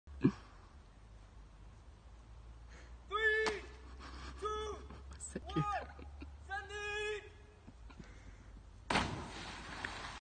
NAKED BRIDGE CLIFF JUMPING P.2
Nice gainer while i was naked, many cars saw me and surely call the cops lmao